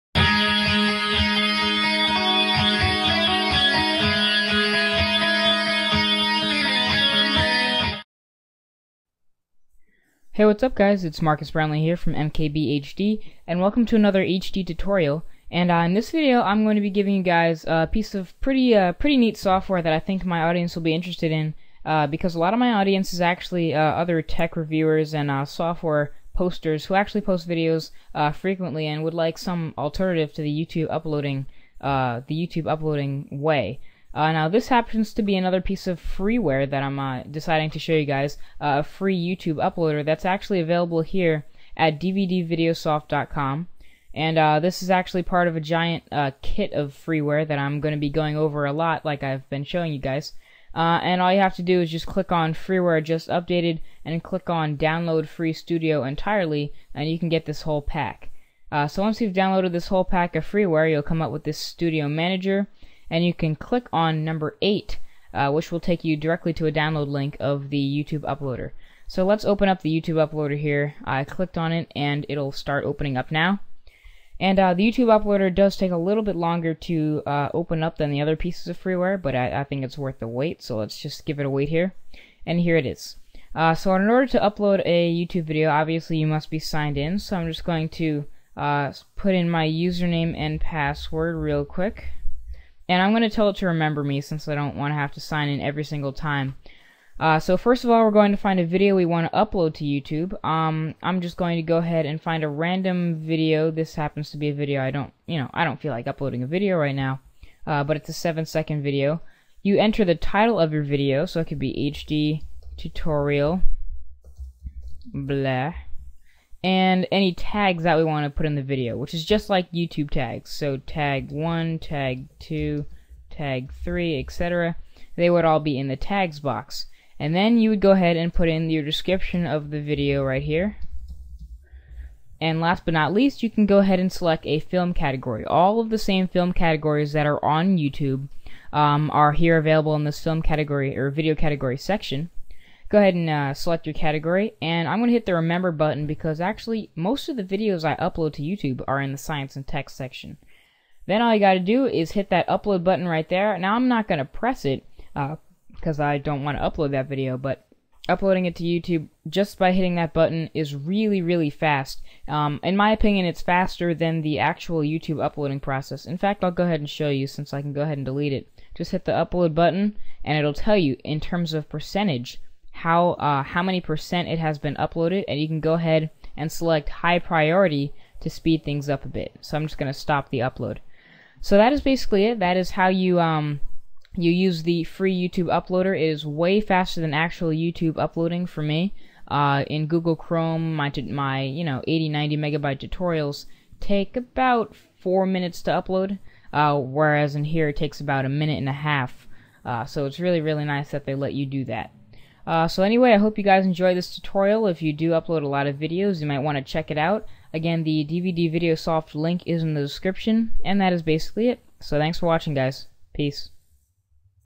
HD Tutorial: Fastest Youtube Uploader
This free Youtube Uploader is the fastest of its kind, and the best part about it? It's freeware! In this video I'll demonstrate signing in, customizing options, inputting the field necessary for uplaoding to Youtube, and finally, the uploading process.